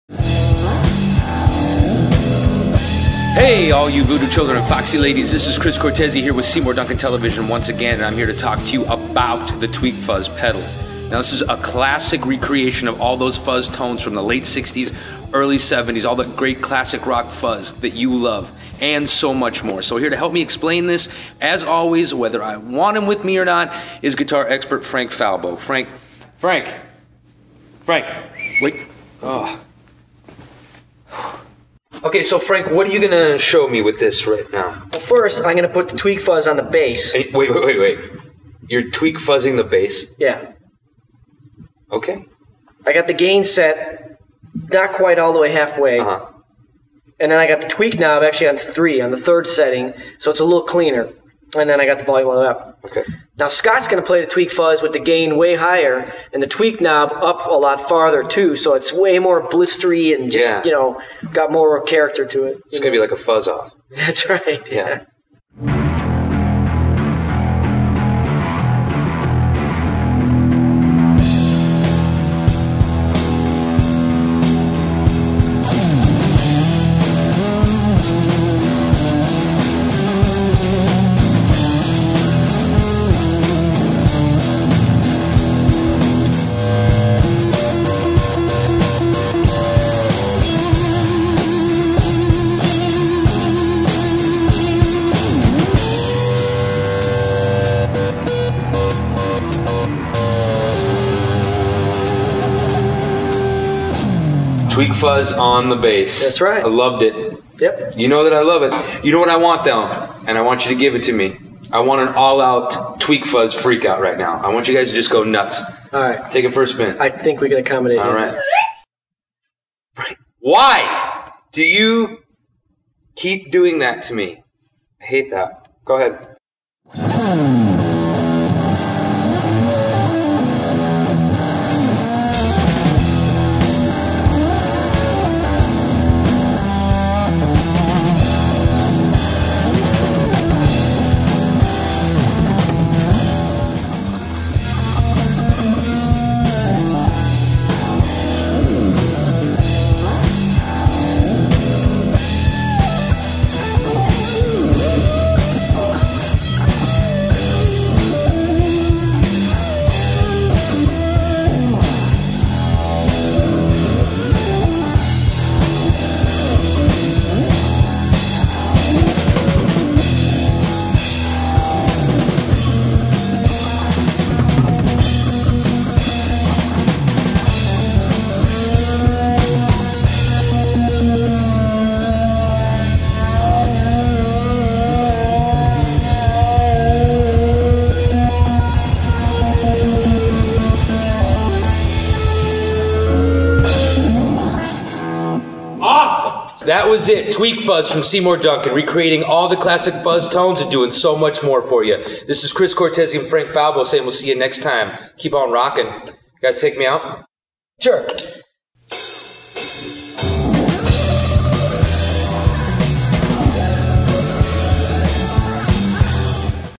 The Tweak Fuzz: Seymour Duncan TV
All the great classic rock fuzz wrapped into one amazing pedal from Seymour Duncan. Whether it's for bass or electric guitar, The Tweak Fuzz can recreate all the gnarly fuzz from the 60's and 70's.